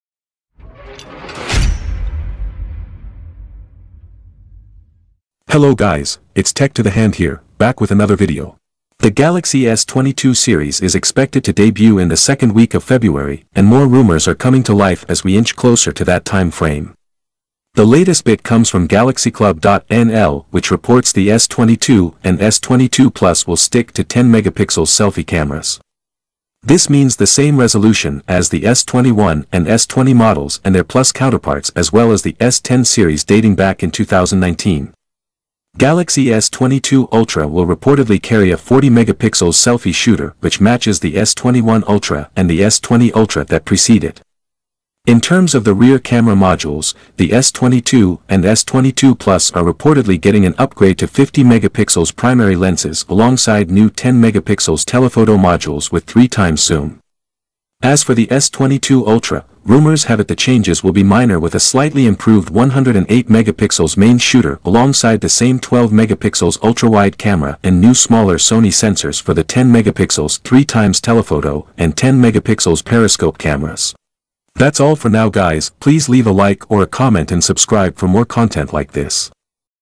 Samsung Galaxy S22 selfie camera "UPGRADE"? What are they waiting for..
The Galaxy S22 series is expected to debut in the second week of February and more rumors are coming to life as we inch closer to that time frame. The latest bit comes from GalaxyClub.nl which reports the S22 and S22+ will stick to 10 Megapixels selfie cameras. This means the same resolution as the S21 and S20 models and their plus counterparts as well as the S10 series dating back in 2019.
Galaxy S22 Ultra will reportedly carry a 40 Megapixels selfie shooter which matches the S21 Ultra and the S20 Ultra that precede it. In terms of the rear camera modules, the S22 and S22+ are reportedly getting an upgrade to 50 Megapixels primary lenses alongside new 10 Megapixels telephoto modules with 3 times zoom. As for the S22 Ultra, rumors have it the changes will be minor with a slightly improved 108 Megapixels main shooter alongside the same 12 Megapixels ultrawide camera and new smaller Sony sensors for the 10 Megapixels 3times telephoto and 10 Megapixels periscope cameras.